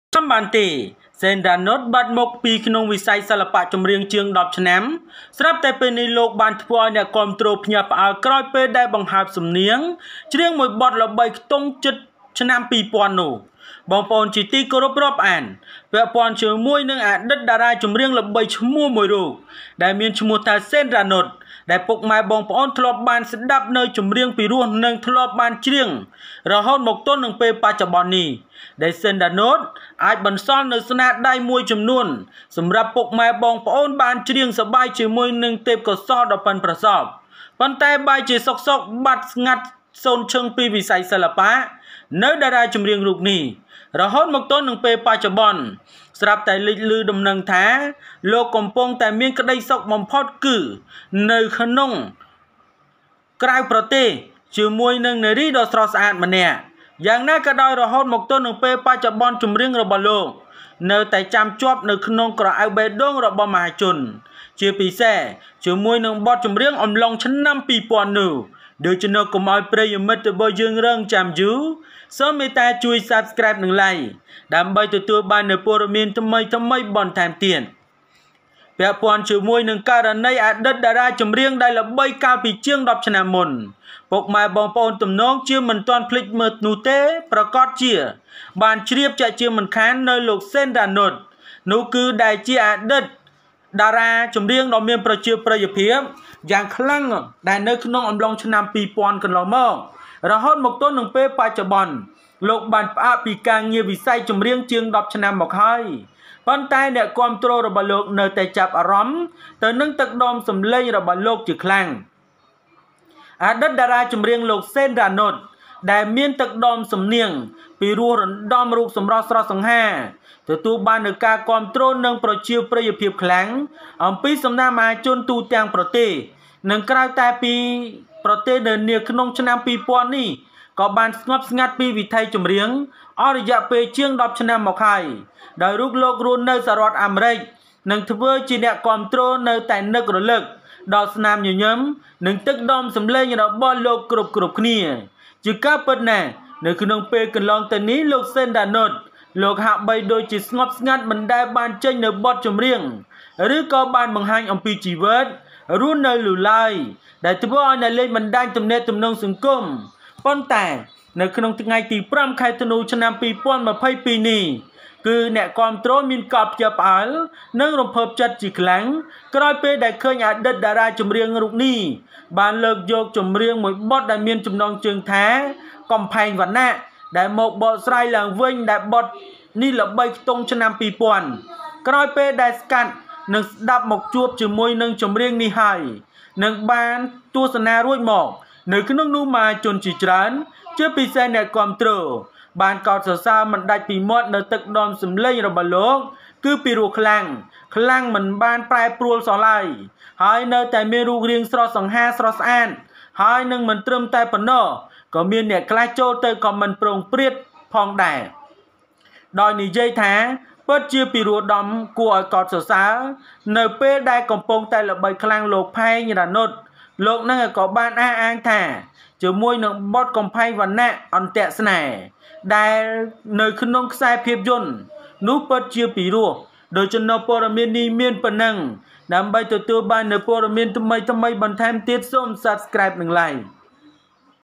នៅចាំបានទេ សេន រ៉ានុត បាត់មុខពីវិស័យចម្រៀងជាង ១០ ឆ្នាំ ស្រាប់តែពេលនេះលោកធ្វើឲ្យអ្នកគាំទ្រភ្ញាក់ផ្អើល
នៅចាំបានទេ សេន រ៉ានុត បាត់មុខពីវិស័យចម្រៀងជាង ១០ ឆ្នាំ ស្រាប់តែពេលនេះលោកធ្វើឲ្យអ្នកគាំទ្រភ្ញាក់ផ្អើល Remember Sen Ranout disappeared from the music industry for more than 10 years, now he surprised fans.